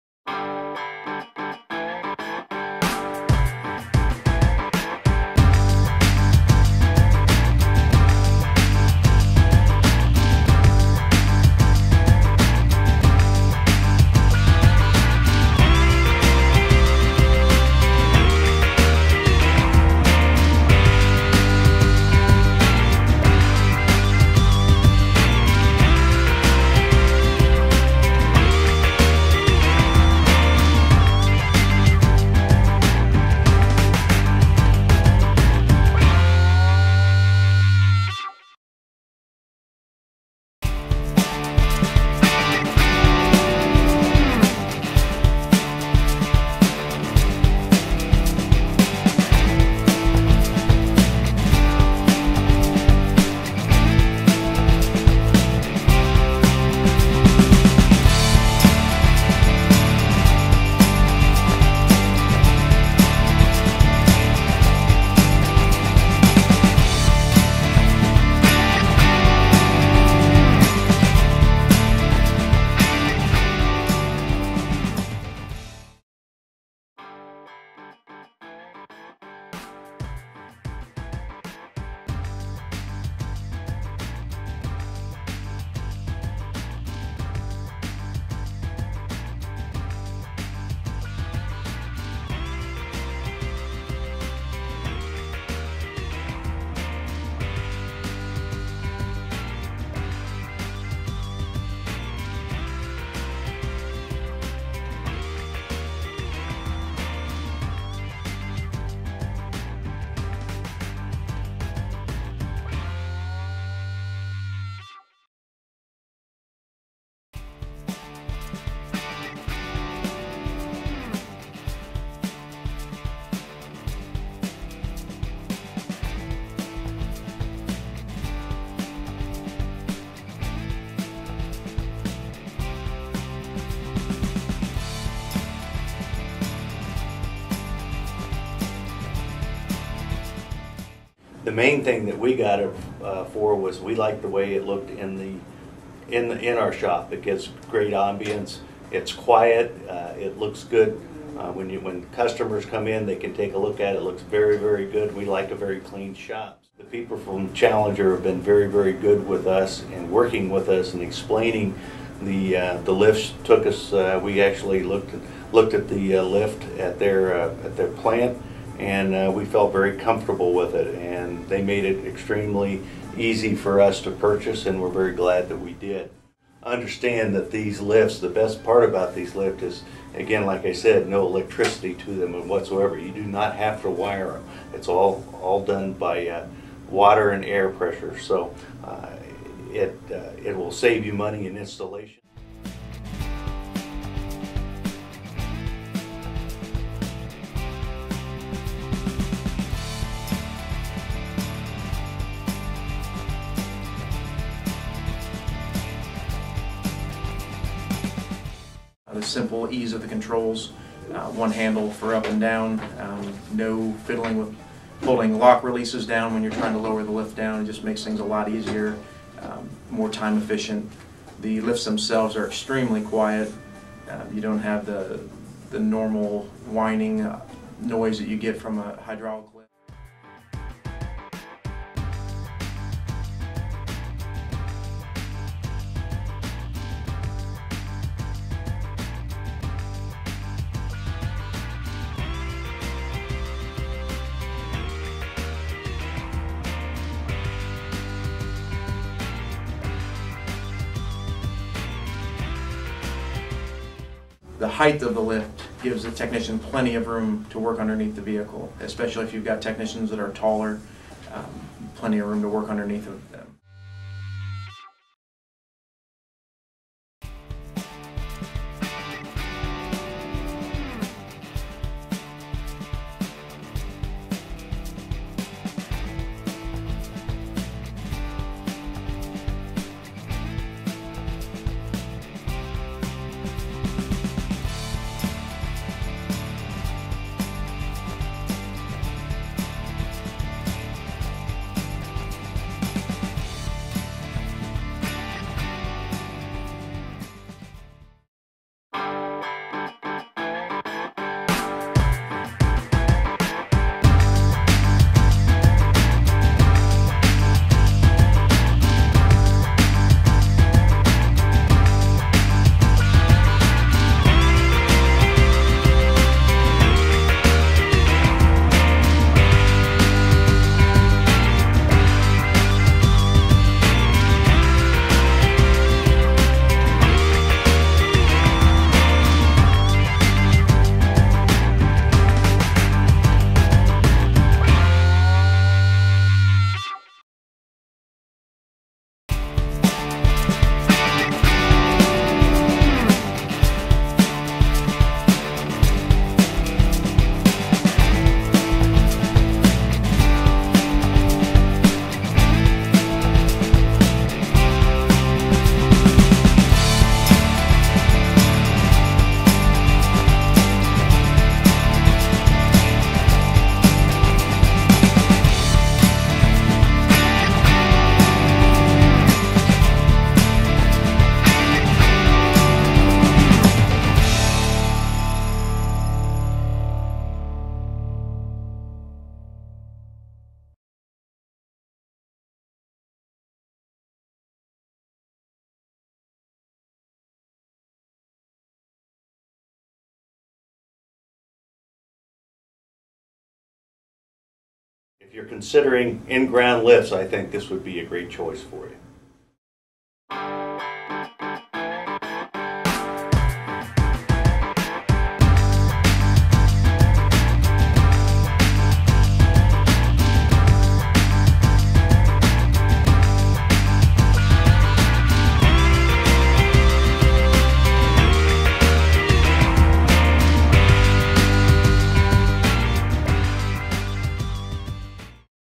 EnviroLift® with AquaVantage™
- The AV1020series EnviroLift® with patented AquaVantage™ Technology uses compressed air and tap water instead of electrical motors and hydraulic oil, making it the one and only realGREEN™ earth friendly product.